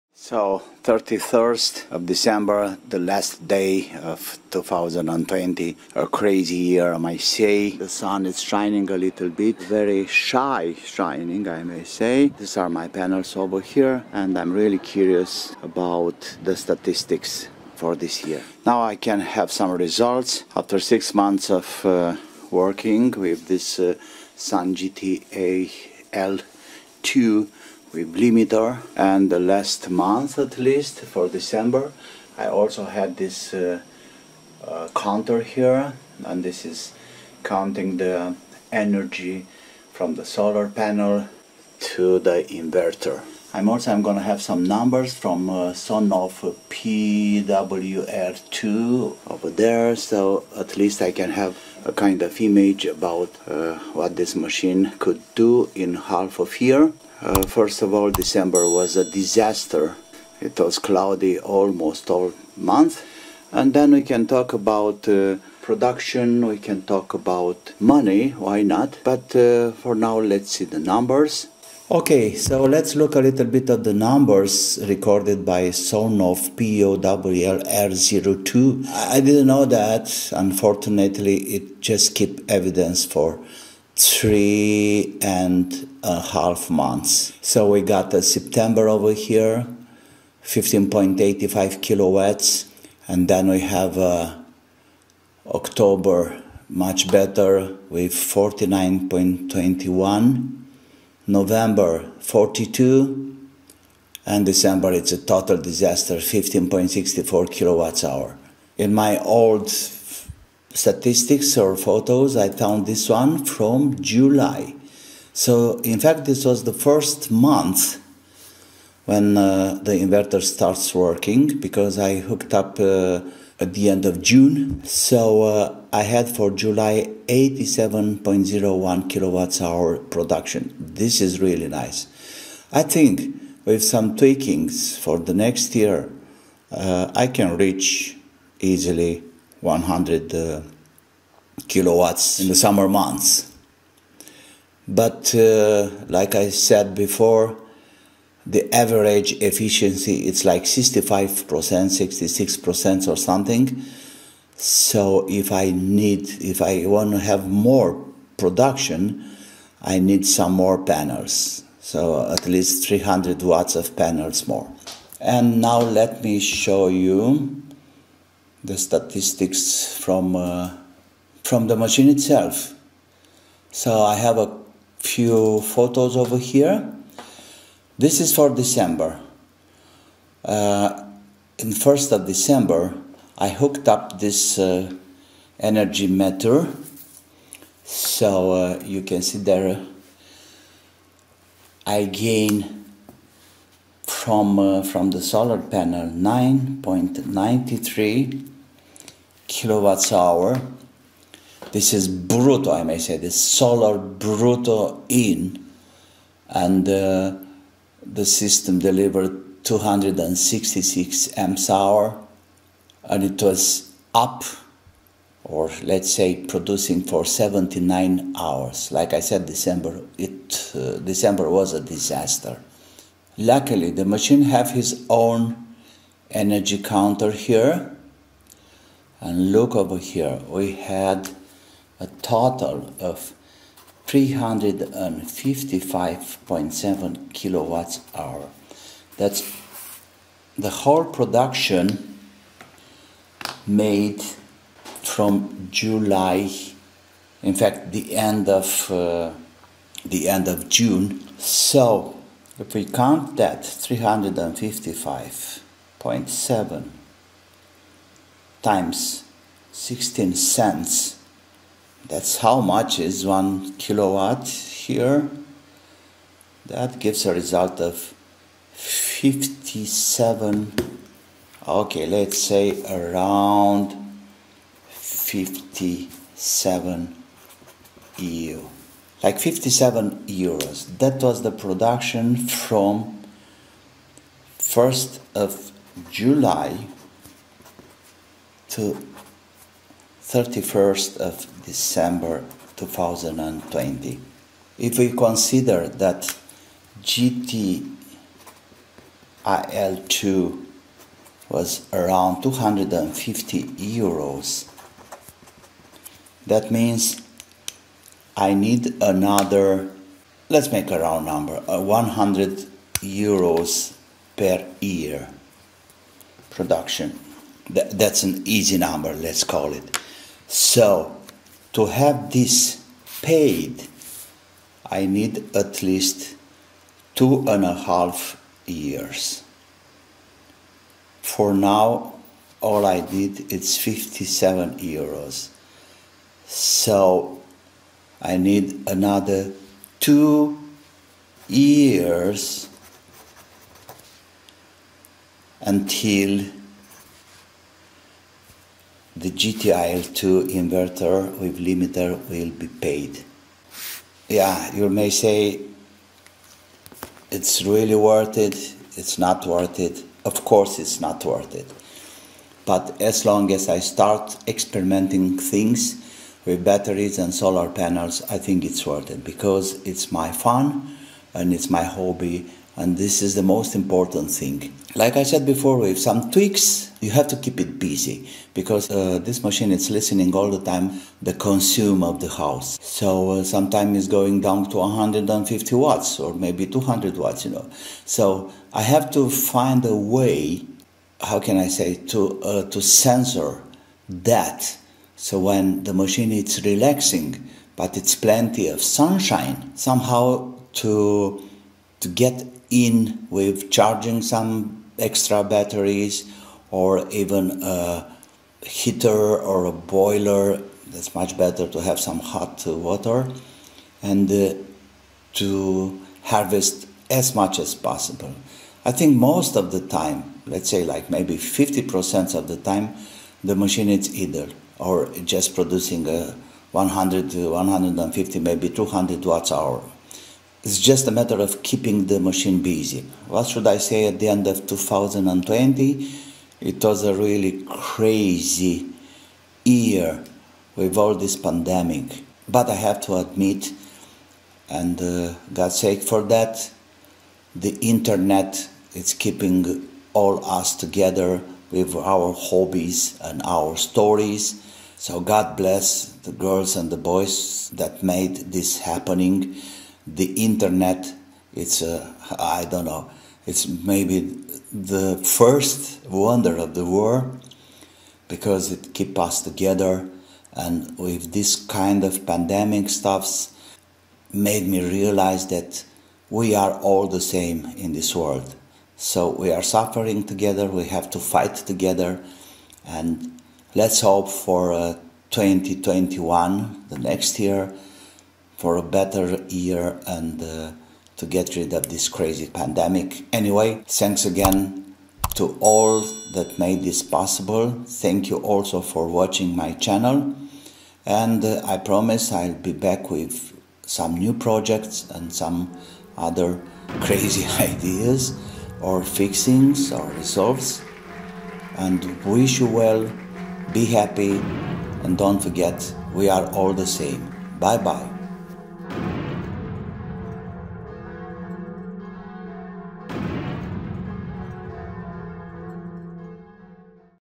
SUN GTIL2 limiter after half year
Some statistics and remarks after half year of production with SUN GTIL2 limiter on grid inverter.Let's talk about money too!
00:00 Intro
01:11 Sonoff statictics
03:10 Energy meter numbers
03:50 SUN GTIL2 statistics
04:12 Let's talk about money